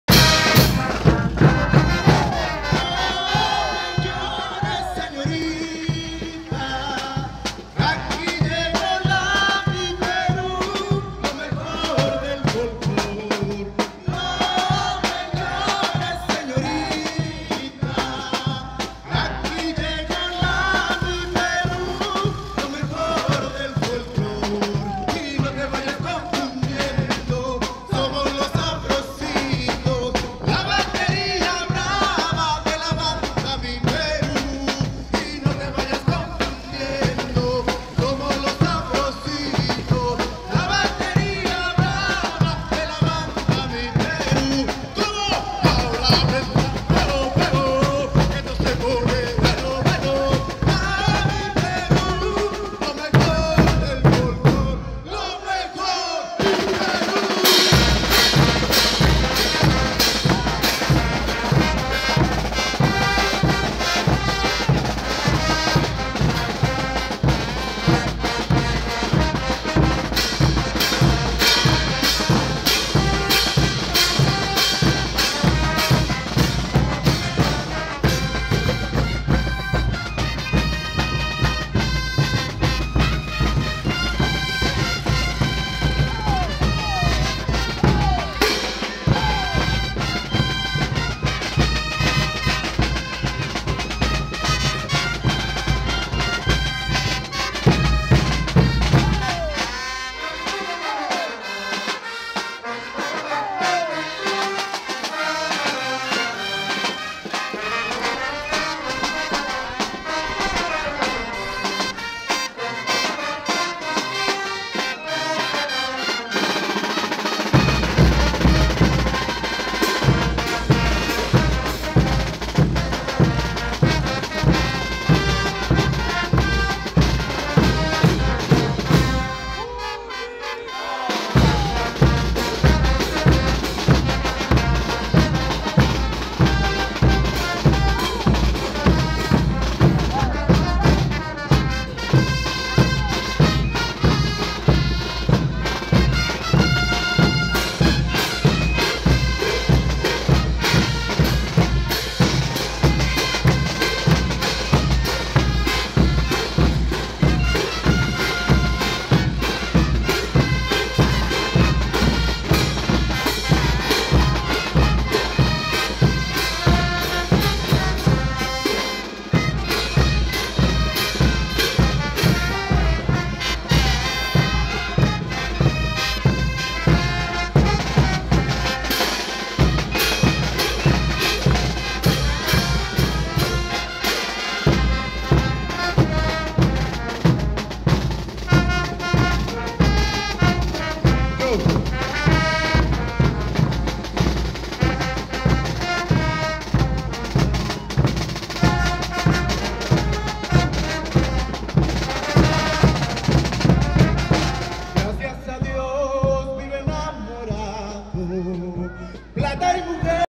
Virgen del rosario de kakachi
Fiesta en honor a la virgen del rosario que realizan la gente de Puno